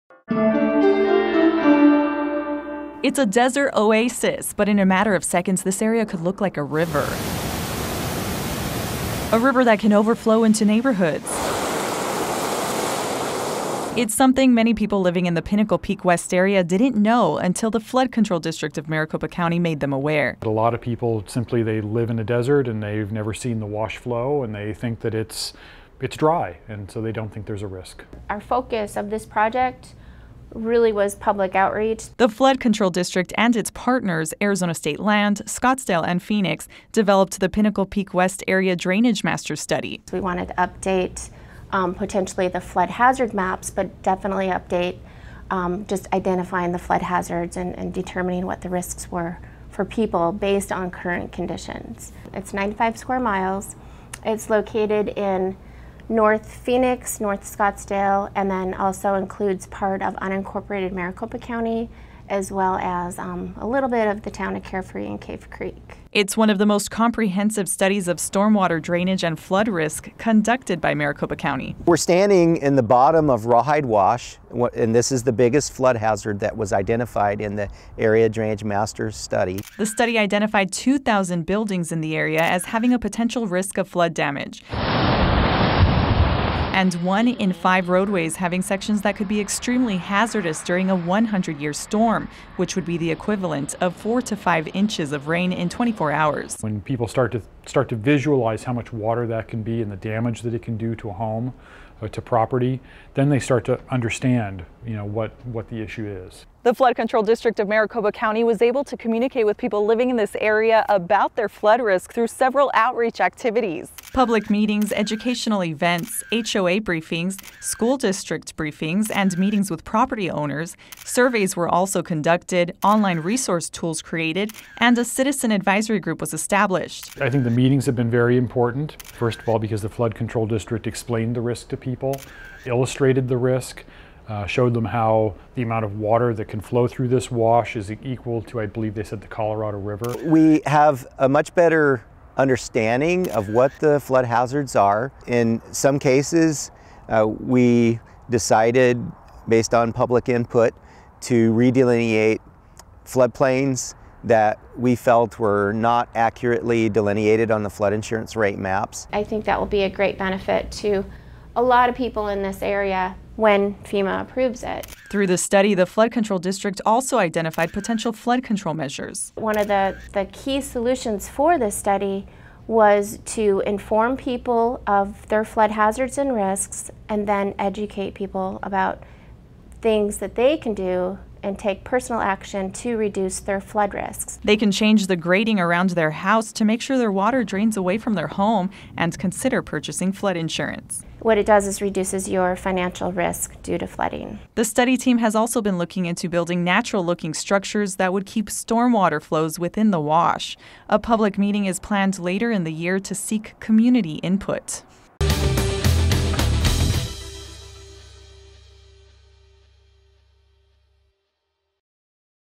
Pinnacle Peak West Area Drainage Master Study
The Pinnacle Peak West (PPW) Area Drainage Master Study (ADMS) is one of the most comprehensive studies of storm water drainage and flood risk conducted by the Flood Control District of Maricopa County (FCD) and its local partners.
The study looks at a 95 square mile area in north Scottsdale and north Phoenix near the Rawhide Wash (Pinnacle Peak & Scottsdale Rds.)
The study helps FCD identify flood hazards in the area of PPW, and regulate, re-mediate and disseminate information to the community about those hazards.